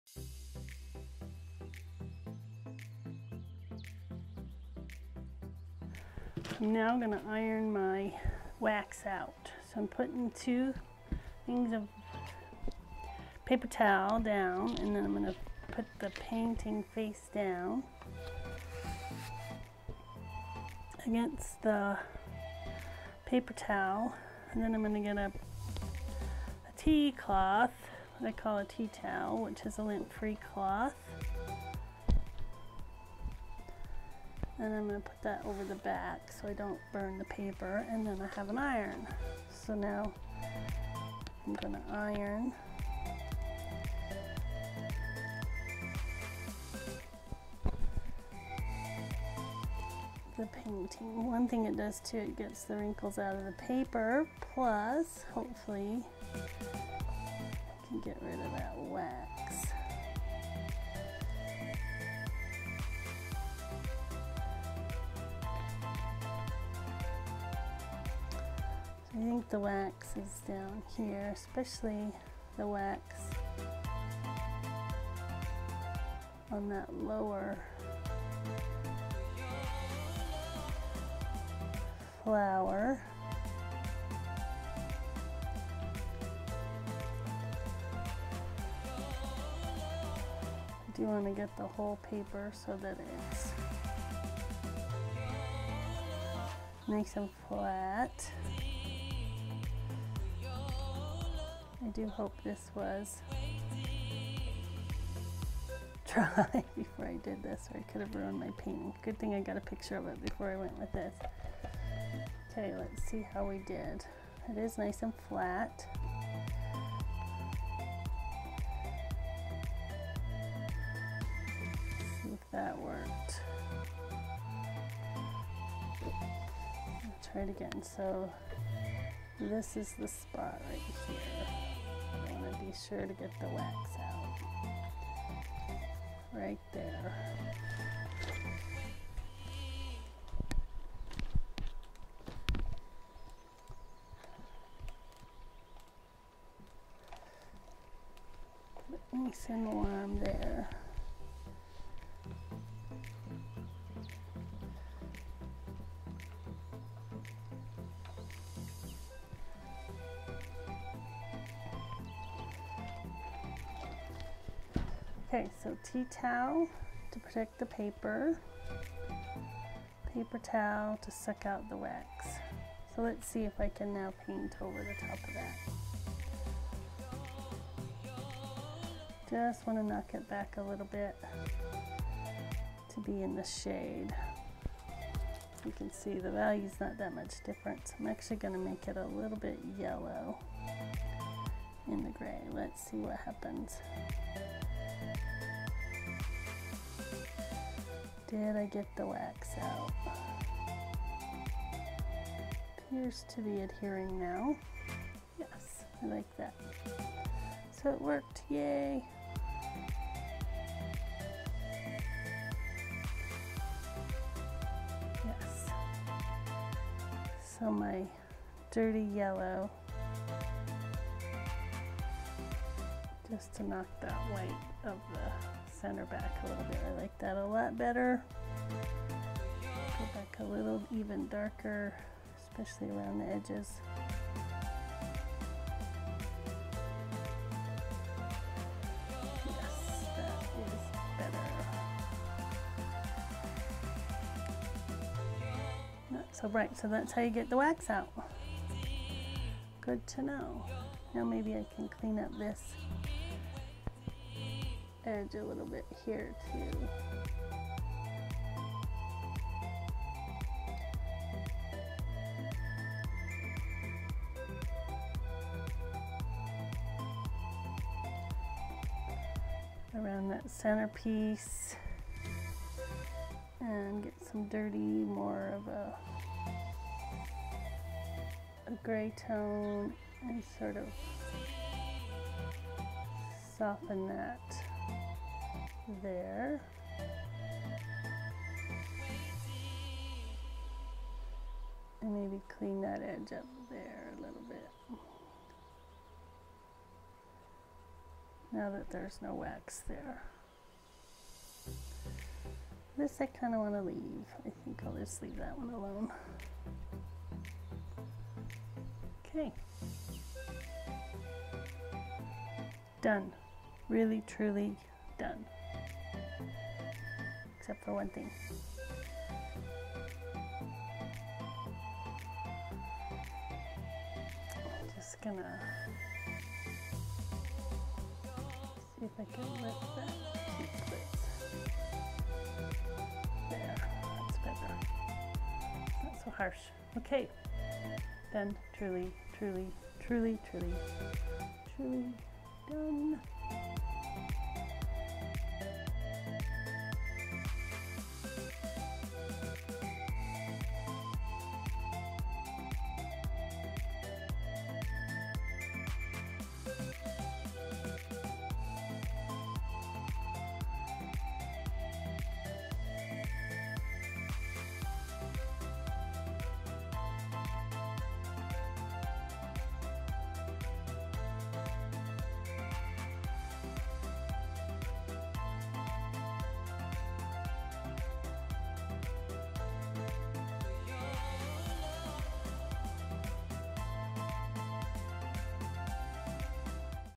Wax Resist Removal
Quick demonstration on how to remove wax resist from a watercolor painting.